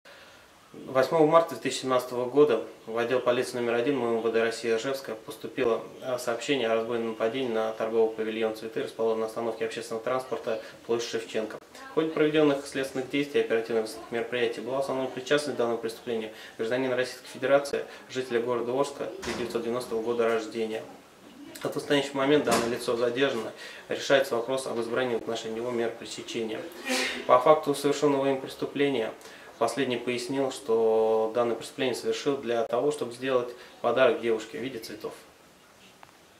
В Орске полицейскими задержан подозреваемый в разбойном нападении на цветочный павильон
Накануне в дежурную часть полиции Орска поступило сообщение о разбойном нападении от продавцов одного из цветочных магазинов.
Около пяти часов утра неизвестный разбил стеклопакет форточного окна остановочного павильона и, угрожая предметом, похожим на пистолет, потребовал отдать ему цветы. Продавец нажала на тревожную кнопку. Злоумышленник скрылся в неизвестном направлении.
В ходе оперативно-розыскных мероприятий оперативники уголовного розыска установили личность подозреваемого. Уже через несколько часов молодой человек был задержан. Им оказался 26-летний ранее судимый местный житель. При личном досмотре злоумышленника оперативники обнаружили предполагаемое орудие преступления – пневматический пистолет.
Подозреваемый не стал отрицать свою причастность к нападению и полностью сознался в содеянном. Он рассказал, что хотел подарить цветы своей девушке.
В отношении молодого человека следователями ОП №1 МУ МВД России «Орское» возбуждено уголовное дело.
В настоящее время подозреваемый задержан в порядке статьи 91 УПК РФ, решается вопрос об избрании в отношении него меры пресечения в виде заключения под стражу.

On the eve of the duty of the police of Orsk received a report of assault from sellers of one of the flower shops.
About five o'clock in the morning unknown persons broke the glass of the window VOSTOCHNOGO of stopping pavilion and, threatening with the subject similar to a pistol, demanded to give him flowers. The seller pressed the alarm button. The attacker fled in an unknown direction.
In a course quickly-search actions field investigators of criminal investigation Department identified the personality of the suspect. After a few hours the young time56.ru man was detained. It was them 26-the summer earlier judged local. At personal inspection of the malefactor field investigators discovered the alleged murder weapon – an air gun.
The suspect did not deny his involvement in the attack and fully confessed to the crime. He said that he wanted to give flowers to his girlfriend.
In relation to the young person investigators of OP No. 1 of MU of the MIA of Russia "Orsk" a criminal case.
At present, the suspect is detained as article 91 UPK the Russian Federation, the question on election concerning it a preventive measure in the form of detention.